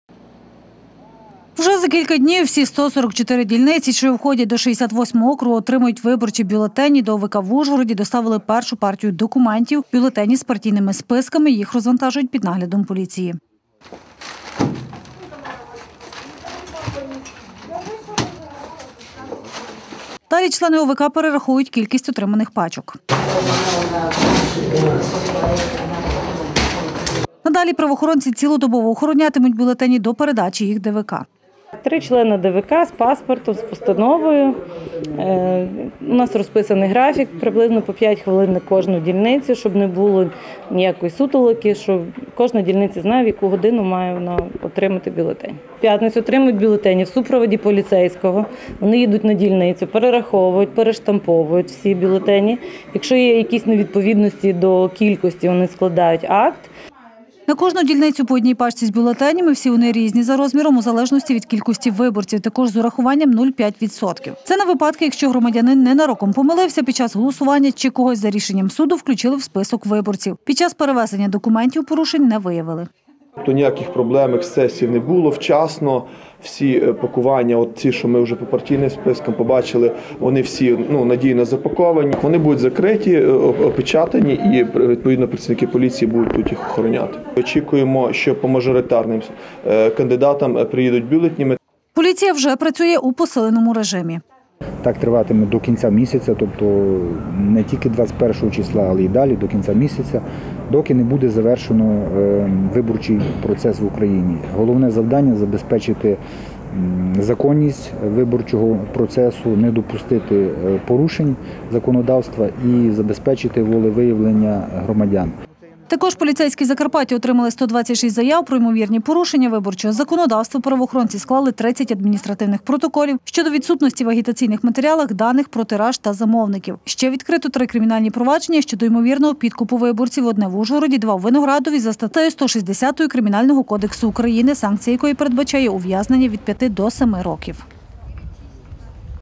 Під охороною
4 з 6 окружних виборчих комісій уже отримали виборчі бюлетені. Згодом всі буде доставлено на 811 ДВК. Після цього дільниці правоохоронці візьмуть під цілодобову охорону.